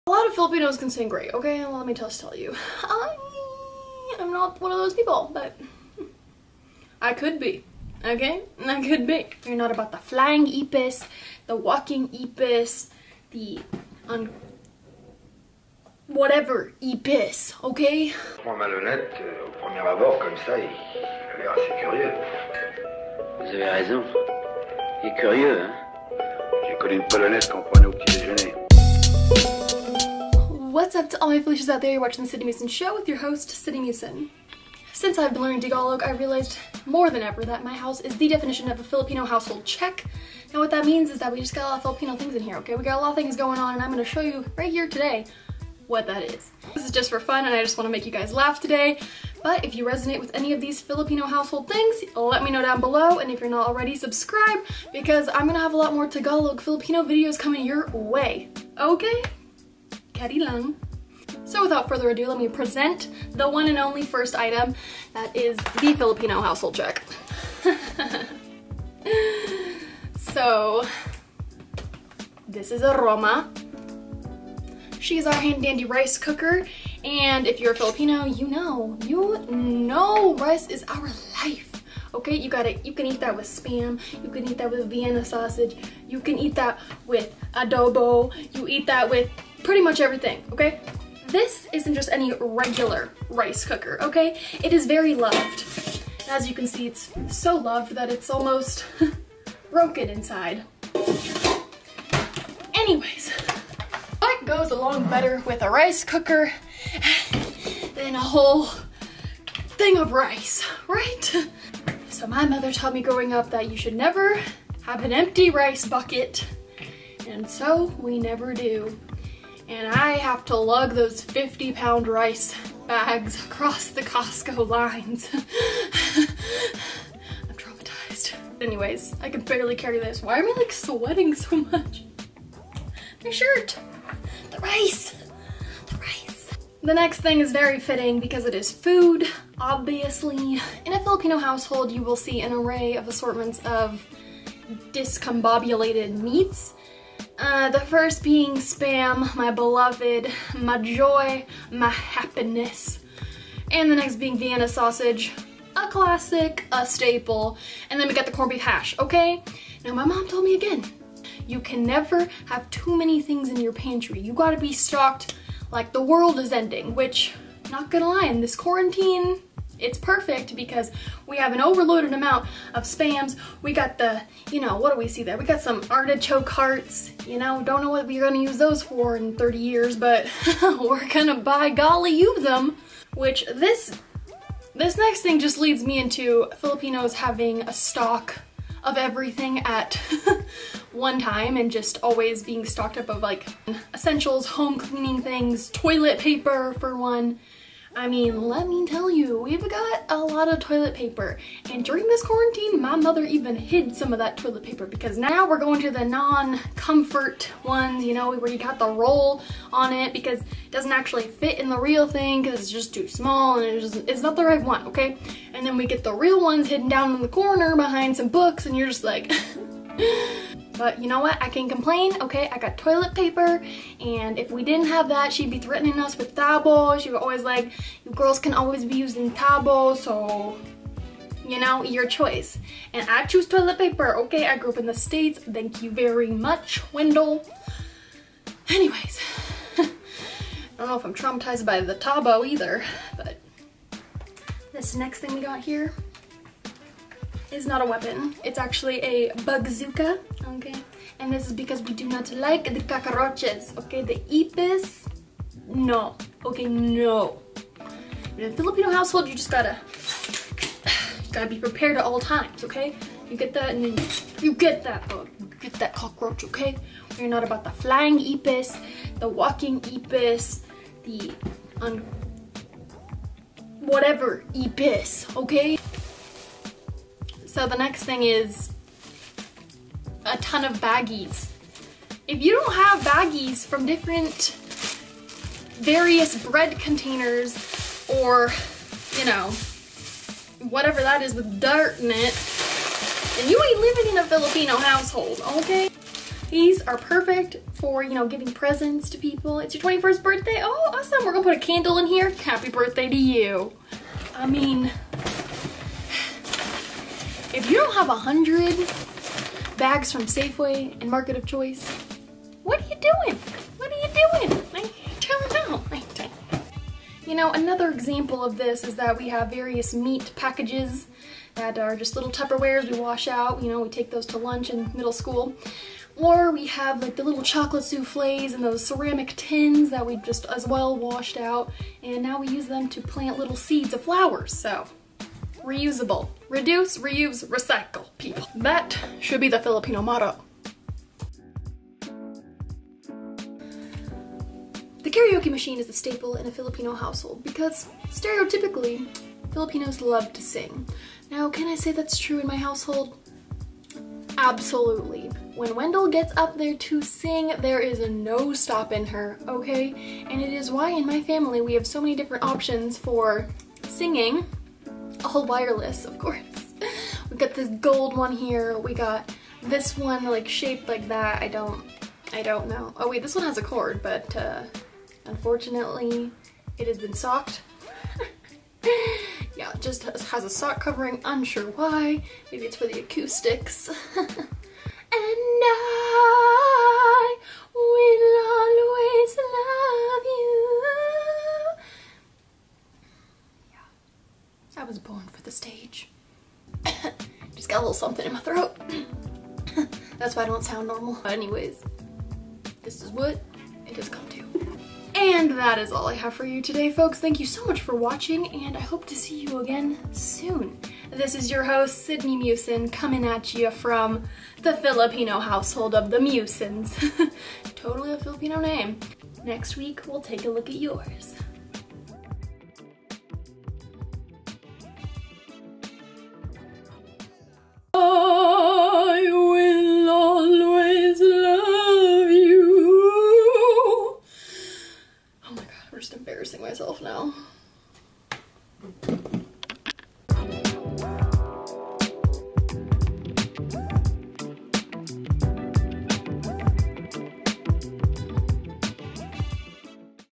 Filipino Household Check ✔️
Hey yo  Filipino Household Check! Today I uncover everything in my house that ultimately solidifies we are in fact a Filipino Household. This is just to make you laugh, so please don't take it too seriously. In my house you will find an array of various meats, lots of rice, and of course a karaoke machine. These are all necessities of a Filipino household, so I hope you enjoy, and I will see you in my next video!
xx Sydney